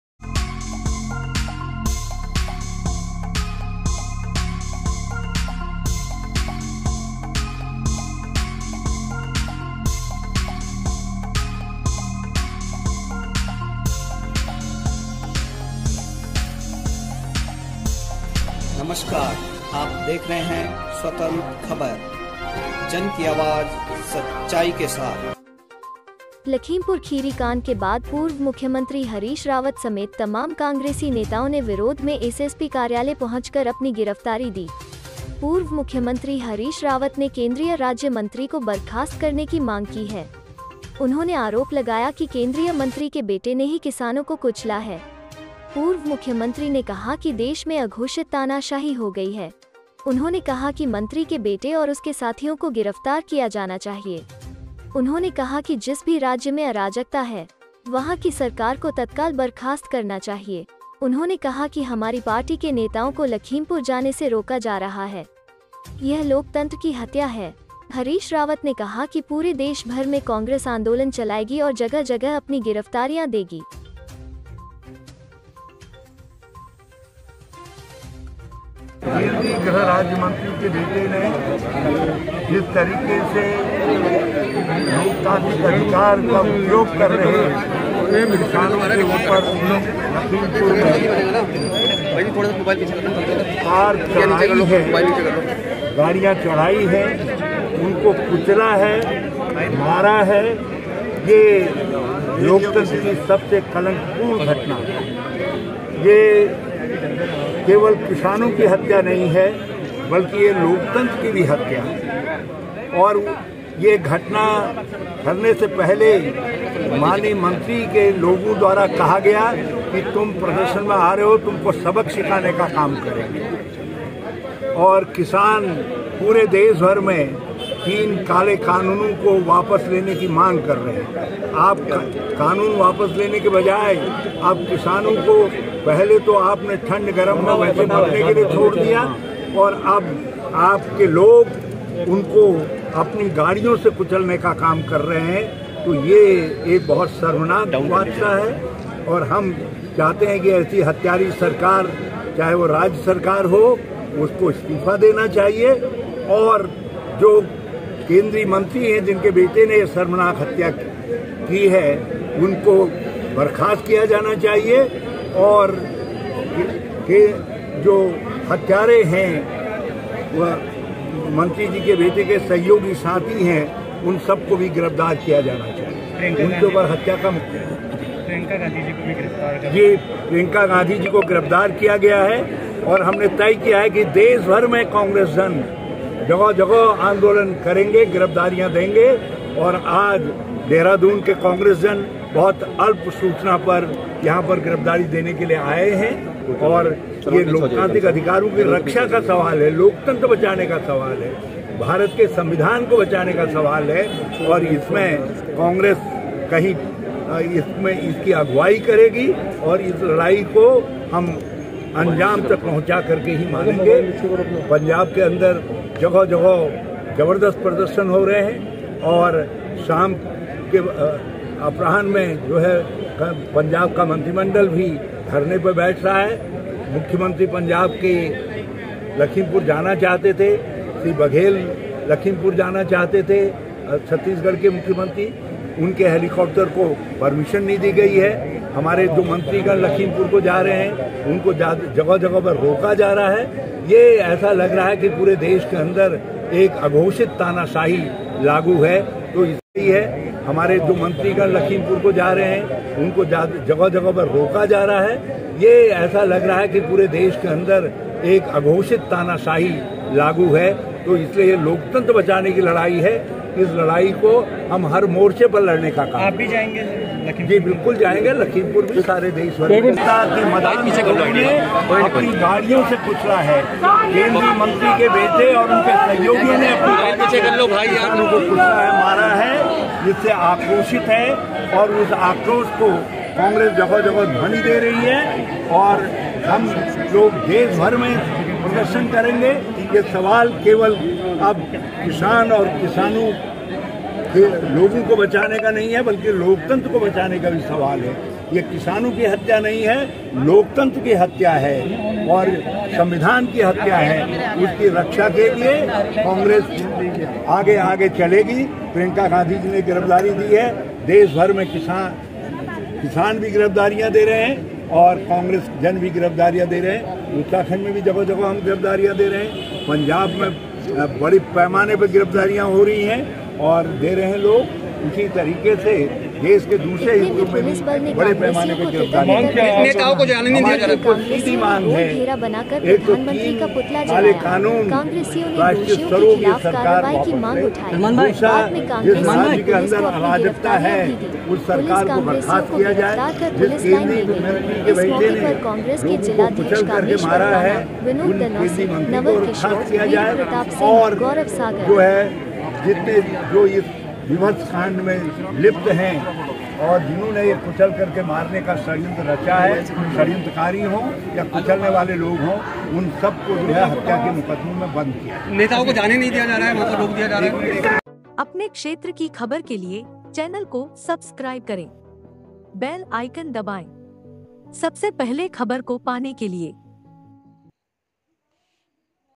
लखीमपुर खीरी कांड के बाद पूर्व मंत्री समेत तमाम नेताओं ने किया विरोध
देहरादून। लखीमपुर खीरी कांड के बाद पूर्व मुख्यमंत्री हरीश रावत समेत तमाम कांग्रेसी नेताओं ने विरोध में एसएसपी कार्यालय पहुंचकर अपनी गिरफ्तारी दी। पूर्व मुख्यमंत्री हरीश रावत ने केंद्रीय राज्य मंत्री को बर्खास्त करने की मांग की है। उन्होंने आरोप लगाया कि केंद्रीय मंत्री के बेटे ने ही किसानों को कुचला है। पूर्व मुख्यमंत्री ने कहा कि देश में अघोषित तानाशाही हो गई है। उन्होंने कहा कि मंत्री के बेटे और उसके साथियों को गिरफ्तार किया जाना चाहिए। उन्होंने कहा कि जिस भी राज्य में अराजकता है। वहां की सरकार को तत्काल बर्खास्त करना चाहिए। उन्होंने कहा कि हमारी पार्टी के नेताओं को लखीमपुर जाने से रोका जा रहा है। यह लोकतंत्र की हत्या है। हरीश रावत ने कहा कि पूरे देश भर में कांग्रेस आंदोलन चलाएगी और जगह-जगह अपनी गिरफ्तारियां देगी।